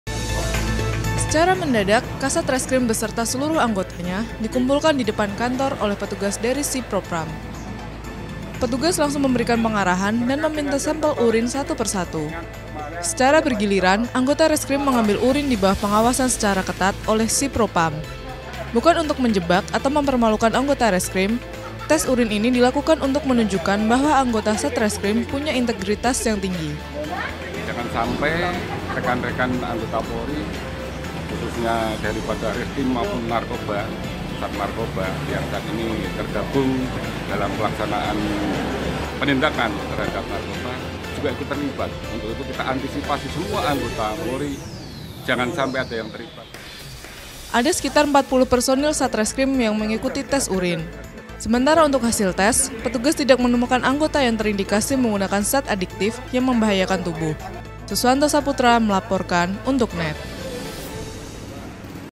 Kasat Reskrim Beserta Anggotanya Mengadakan Tes Urin Mendadak - NET JATENG
Secara mendadak, kasat reskrim beserta seluruh anggotanya, dikumpulkan di depan kantor, oleh petugas dari sie propam, petugas langsung memberikan pengarahan, dan meminta sampel urine satu persatu.

Secara bergiliran, anggota reskrim mengambil urin, dibawah pengawasan secara ketat, oleh sie propam. Bukan untuk menjebak atau mempermalukan anggota reskrim, tes urin ini dilakukan untuk menunjukan bahwa anggota sat reskrim punya integritas yang tinggi.

“jangan saampai rekan2 dari polri reskrim  maupun narkoba sat narkoba dalam pelaksanakan penindakan terhadap juga jangan terlibat jangan sampai ada yang terlibat.” IPTU. MUJIHANDARI - Kasie Propam Polres Grobogan

Ada sekitar empat puluh personil sat reskrim yang mengikuti tes urin. Sementara untuk hadil tes, petugas tidak menemukan anggota yang terindikasi menggunakan zat aditif yang membahayakan tubuh.